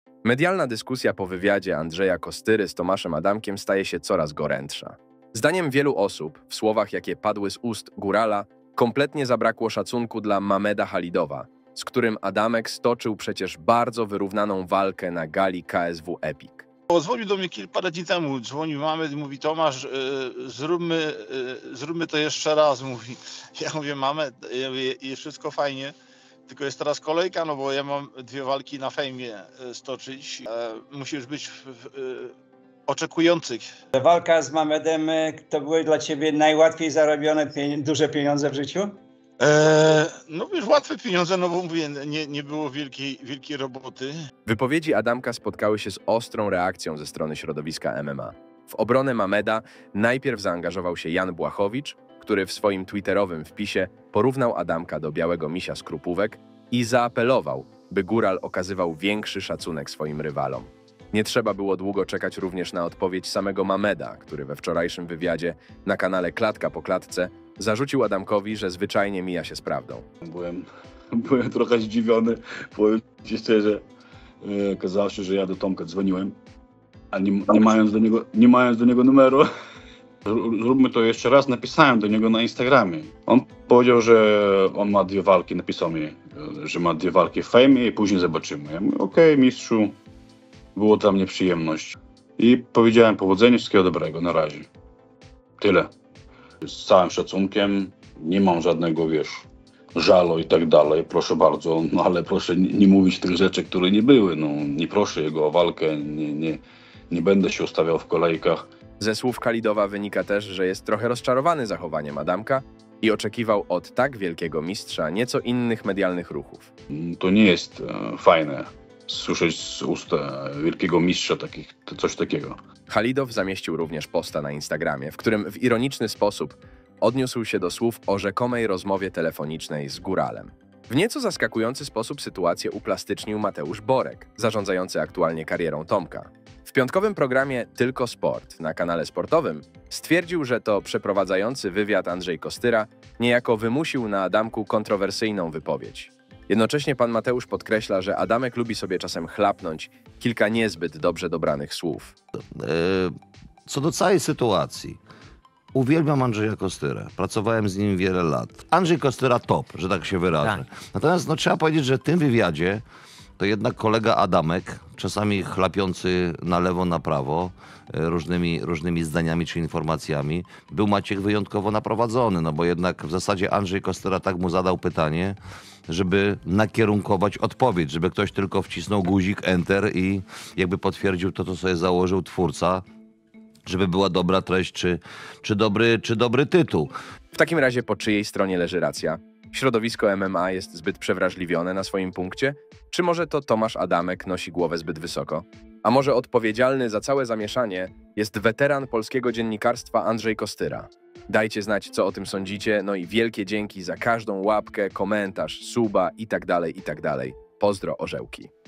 KHALIDOV UJAWNIA KŁAMSTWO ADAMKA, a BOREK OBWINIA KOSTYRĘ! (FAME MMA, BŁACHOWICZ, KSW)
Mamed Khalidov odniósł się do wywiadu, jakiego Tomasz Adamek udzielił Andrzejowi Kostyrze na na kanale KOstyraSE. Legenda KSW zarzuciła kłamstwo podopiecznemu Mateusza Borka, który aktualnie prowadzi karierę Tomasza i doprowadził do podpisania przez "Górala" kontraktu z federacją Fame MMA, gdzie już w maju odbędzie się walka Adamek - Bandura.

Khalidov nie pozostał dłużny Adamkowi, którego postawa wzbudziła gorącą dyskusję w środowisku polskich sportów walki i spowodowała, że "odpaliła się" nawet ikona MMA w Polsce Jan Błachowicz. Poza Błachowiczem swoje trzy grosze wtrącił też dziś Mateusz Borek, który wykorzystał Kanał Sportowy i program "Tylko Sport" (cóż za przeciwieństwo do Kanału Zero), aby obronić Tomasza Adamka i podkreślić, że "Góral" nie udzieliłby tak kontrowersyjnej wypowiedzi, gdyby nie "nakierował" go na nią przeprowadzający wywiad Andrzej Kostyra.

Mamed Chalidow w wywiadzie na kanale "Klatka po Klatce" zaprzeczył jakoby dzwonił do Adamka po walce na KSW Epic i prosił się o rewanż, a cała interakcja obu panów miała być jedynie kurtuazyjną wymianą wiadomości na Instagramie. Mamed sugeruje więc, że Adamek w swoich medialnych wypowiedziach kłamie, a z pewnością mija się z prawdą prezentując swój punkt widzenia na relację z gwiazdą KSW. Ponadto Khalidov dodał post na Instagramie, w którym prześmiewczo odnosi się do słów Adamka o rzekomej rozmowie oraz kolejce chętnych, do której musiałby ustawić się Mamed w oczekiwaniu na rewanżową walkę z Adamkiem . Przy okazji warto tutaj wspomnieć, że dziś odbywa się ważenie przed galą XTB KSW 92, w której udział wezmą m.in. Jakub Wikłacz i Michał Materla.

Oprócz Błachowicza, który zamieścił na Twitterze krytykujący Adamka wpis, w którym porównał go do Białego Misia z Krupówek i wezwał do okazywania większego szacunku Mamedowi Khalidovowi i innym zawodnikom, głos w sprawie zabrał również Mateusz Borek. Na Kanale Sportowym komentator podkreślił, że bardzo szanuje Andrzeja Kostyrę i jego osiągnięcia, jednak to właśnie weteran polskiego dziennikarstwa może być po części odpowiedzialny za słowa, jakie padły z ust Tomasza Adamka.

Po wywiadzie Kostyry z Adamkiem zawrzało m.in. dlatego, iż z ust Pana Andrzej padło pytanie "czy były to najłatwiej zarobione pieniądze w życiu" Adamka. Taka konstrukcja zapytania miałaby sprawić, że Tomasz nie miał innego wyboru jak potwierdzić, a tym samym niejako nie okazać należytego szacunku Mamedowi Chalidowowi. Czy cała gorąca dyskusja to początek konfliktu Adamek - Khalidov, a może również Błachowicz - Adamek i tworzenie podłoża pod ewentualną walkę na KSW Epic? Pamiętajmy, że w kontrakcie na walkę Khalidov - Adamek istniał zapis mówiący o tym, że ewentualny pojedynek rewanżowy może odbyć się tylko pod banderą federacji Fame MMA.

Jak oceniacie medialne zamieszanie, w którym uczestniczy Mamed Khalidov, Tomasz Adamek, Jan Błachowicz, Mateusz Borek,  a nawet Andrzej Kostyra? Czy środowisko polskich sportów walki wyczuło okazję na wzięcie "przykładu" z freaków i zrobienie medialnych "dymów", aby w przyszłości móc spieniężyć je w oktagonie jednej z organizacji. Czy rację ma Mateusz Borek lekko zrzucający winę za wypowiedzi Tomasza Adamka na swojego kolegę po fachu Andrzeja Kostyrę? Czy Khalidov odpowiadając Adamkowi i twierdząc, że bokser mija się z prawdą ma gdzieś z tyłu głowy , że gdyby nie kontuzja, walka mogła potoczyć się na jego korzyść? No, a może to po prostu Tomasz Adamek robi sobie rozgrzewkę przed wejściem do świata Fame MMA i celowo prowokuje byłego rywala oraz polaryzuje publiczność, aby jego nazwisko odmieniane było w mediach przez wszystkie przypadki, a jego walka z Bandurą okazała się sprzedażowym sukcesem?

Dajcie znać co myślicie o konflikcie Adamek - Khalidov po ich walce na KSW Epic i czy chcielibyście zobaczyć to starcie na Fame MMA. No, a może Jan Błachowicz i Mamed Khalidov zmieniający się co rundę, a po drugiej stronie wyjaśniający ich Tomasz Adamek? :)

Z góry dzięki za każdą łapkę, komentarz i suba,

Pozdro Orzełki, auuu!